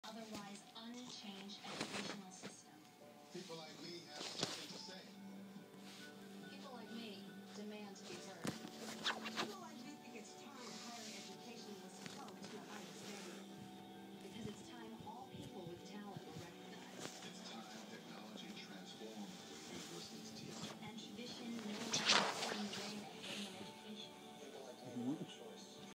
Sphynx Cat Falls Off Bed
Conor was attacking my hand and the covers.. Then he fell off the bed..he was pretty embarassed..